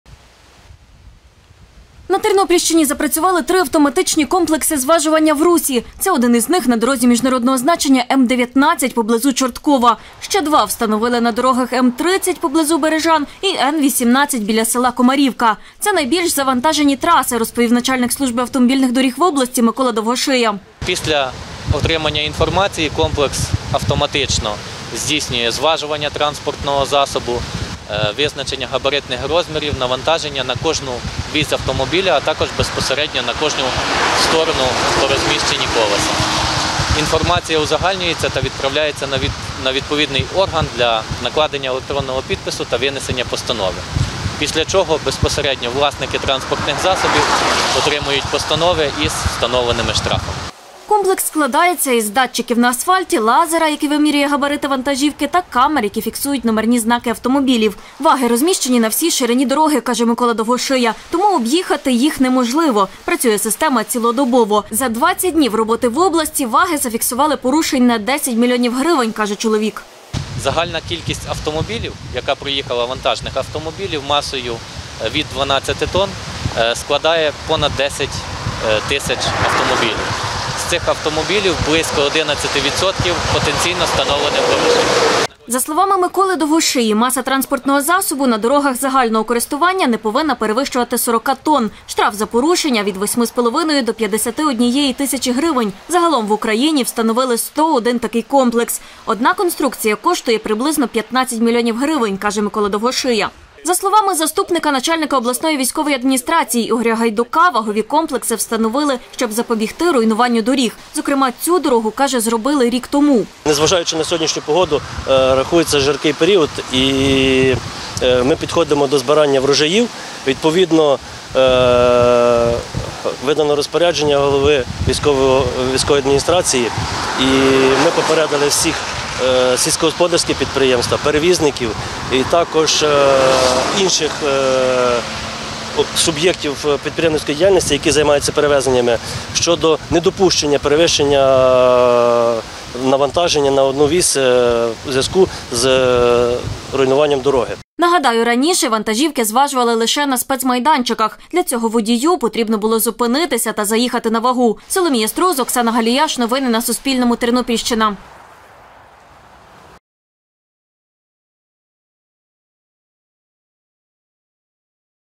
На дорогах Тернопільщини запрацювали ваги, які визначають масу вантажівок під час руху
10 мільйонів гривень штрафу менш, ніж за місяць. На дорогах Тернопільщини запрацювали ваги, які визначають масу вантажівок під час руху. Їх встановили на трьох дорогах. Порушення фіксують автоматично, потім людині приходить штраф.